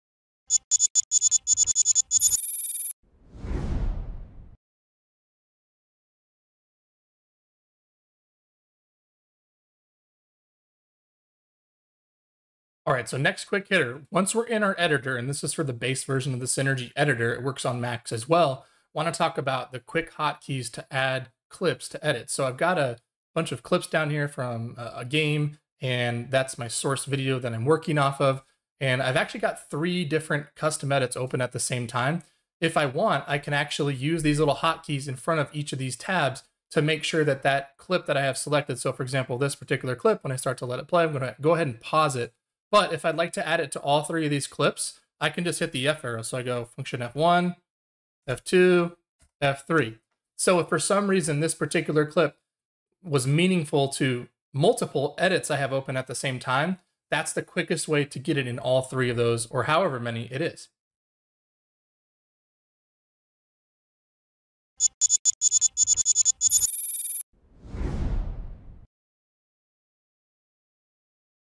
Adding Clips to Multiple Edits in the Synergy Editor
In this video we demonstrate how to add a clip to multiple edits using the hotkeys in the Synergy Editor.

To learn more about Synergy Sports, go to synergysports.com

If you are a High School coach and want more information on our products, use the link below to reach out to a Synergy representative

Chapters:
00:00 Intro
00:22 Using Hotkeys to Add Clips to Multiple Edits
00:50 Outro